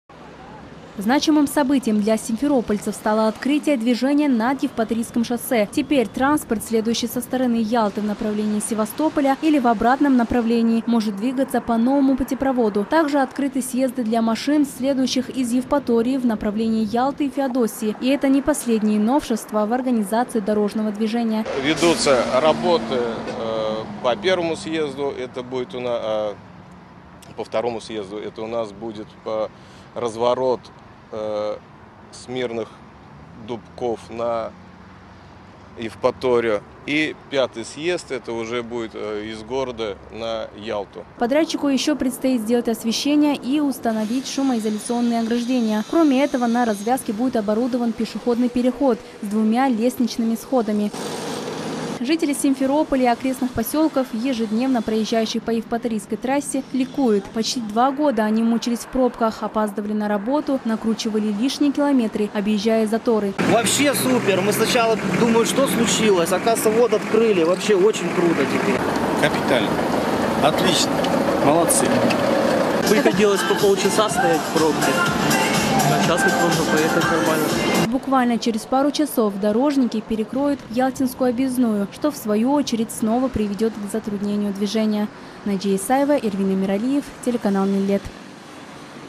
По путепроводу над Евпаторийским шоссе открылось движение
Это не миф, а реальность. Открылось движение по путепроводу над Евпаторийским шоссе. О том, как теперь будут ездить автомобили и уменьшились ли пробки на дороге, расскажем в нашем следующем сюжете.